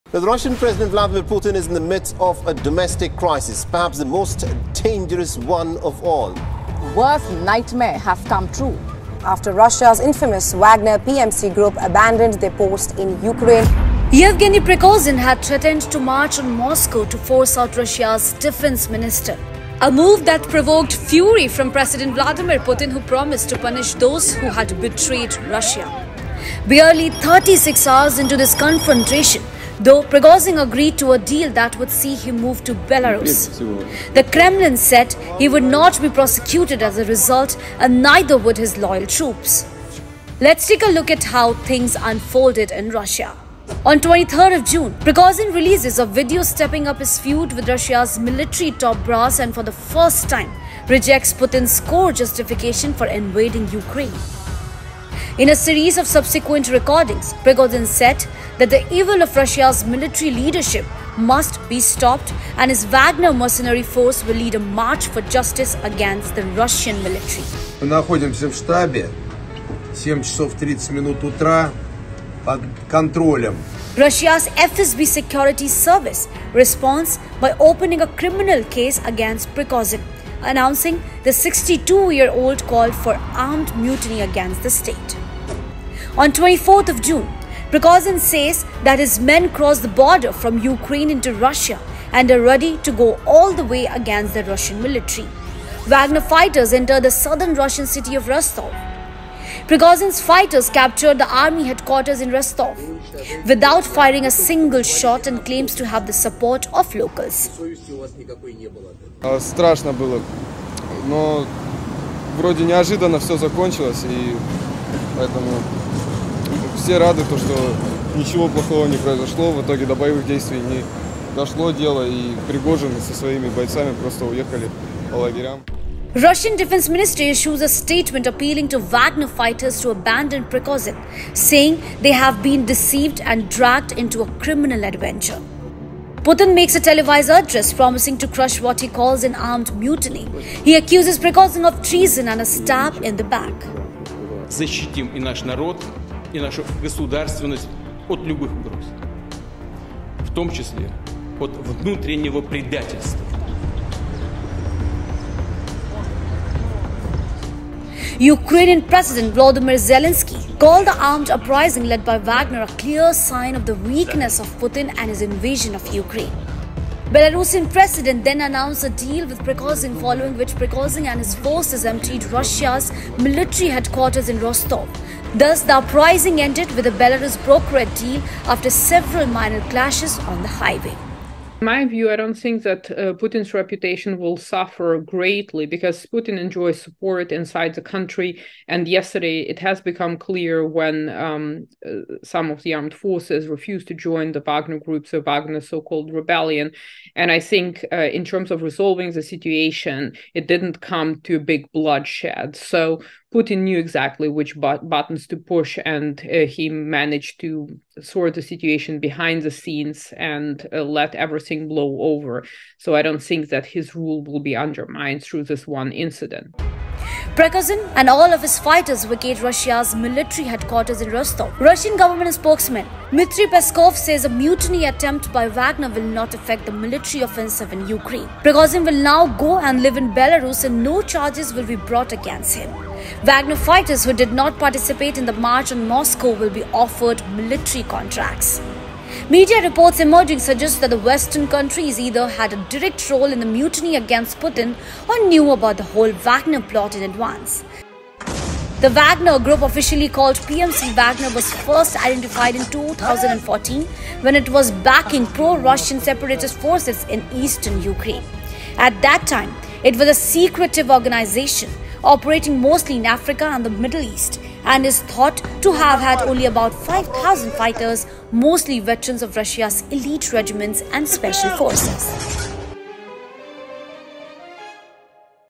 Wagner Group Mutiny: 36 Hours And Hundreds Of Kilometres Of Rebellion Unfolded
The mutiny formed by the Wagner militia company, initially working on the directions of Russian President Vladimir Putin, shook up the nation posing a threat to the current government. The mutiny, though broken in just a span of one day, certainly prompted panic in the Kremlin.